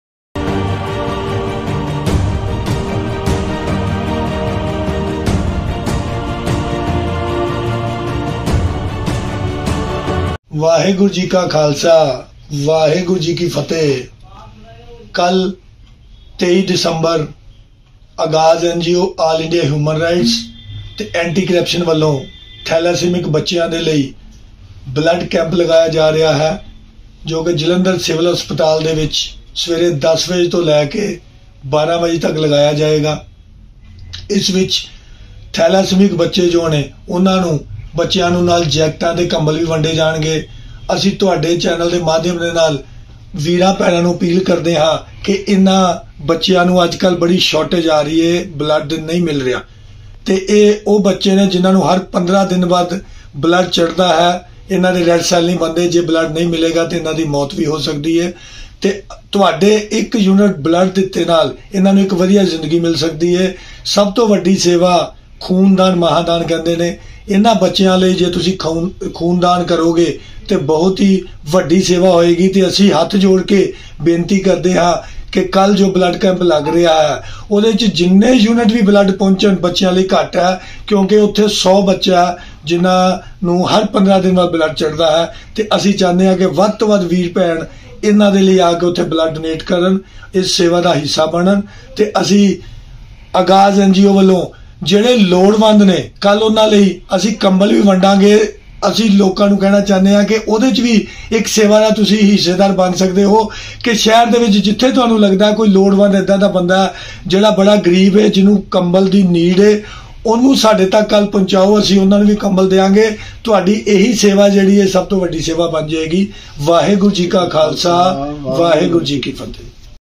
ਆਗਾਜ਼ NGO ਵੱਲੋਂ ਚਾਰ ਸਾਹਿਬਜ਼ਾਦਿਆਂ ਸ਼ਹੀਦੀ ਨੂੰ ਸਮਰਪਿਤ ਥੈਲਾਸੀਮਕ ਬੱਚਿਆਂ ਸਿਵਲ ਬੱਲਡ ਬੈਂਕ ਚ ਖ਼ੂਨ ਦਾਨ ਕੈਂਪ
ਆਗਾਜ਼ NGO ਵੱਲੋਂ ਚਾਰ ਸਾਹਿਬਜ਼ਾਦਿਆਂ ਦੀ ਸ਼ਹੀਦੀ ਨੂੰ ਸਮਰਪਿਤ ਥੈਲਾਸੀਮਕ ਬੱਚਿਆਂ ਲਈ ਸਿਵਲ ਹਸਪਤਾਲ ਜਲੰਧਰ ਬੱਲਡ ਬੈਂਕ ਚ ਖ਼ੂਨ ਦਾਨ ਕੈਂਪ ਲਗਾਇਆ ਜਾ ਰਿਹਾ ਹੈ